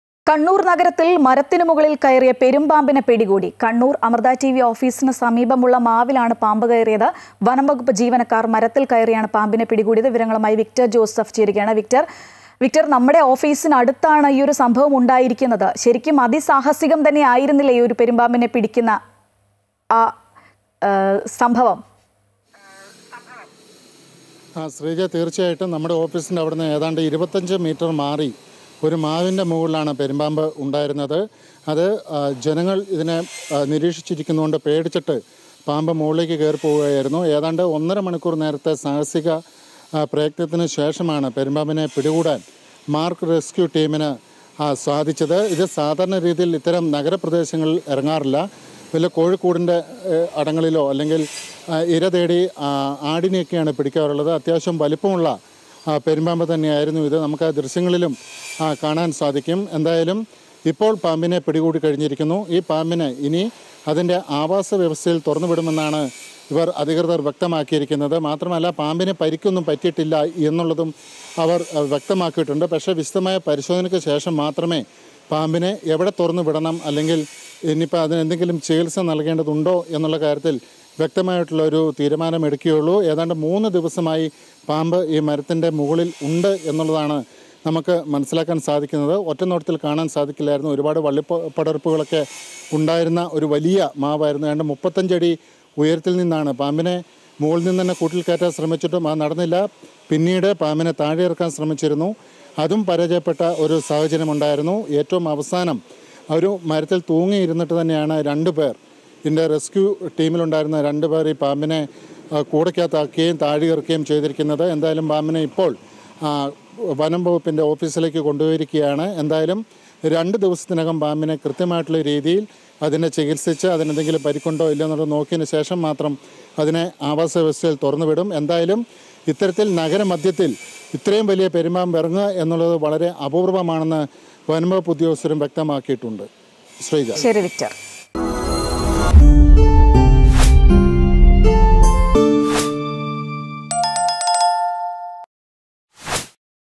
കണ്ണൂർ നഗരത്തിൽ മരത്തിന് മുകളിൽ കയറിയ പെരുമ്പാമ്പിനെ പിടികൂടി | Amrita News
കണ്ണൂർ നഗരത്തിൽ മരത്തിന് മുകളിൽ കയറിയ പെരുമ്പാമ്പിനെ പിടികൂടി. കണ്ണൂർ അമൃത ടി വി ഓഫീസിന് സമീപമുള്ള മാവിലാണ് പാമ്പ് കയറിയത്. വനം വകുപ്പ്  ജീവനക്കാർ മരത്തിൽ കയറിയാണ് പാമ്പിനെ പിടികൂടിയത്..